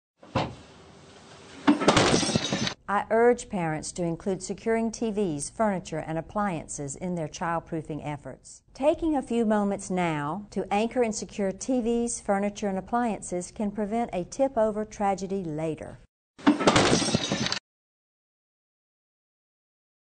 FURNITURE TIPOVER PSA - FURNITURE
I can still remember — in horrifying slow motion — being 4 years old and deciding to climb my dresser like the dummy shown in the video. Fortunately the only casualty was a creepy clown lamp, but it wasn't a mistake I made twice. I anchor major furniture in my house, but I'm not sure I'm doing enough.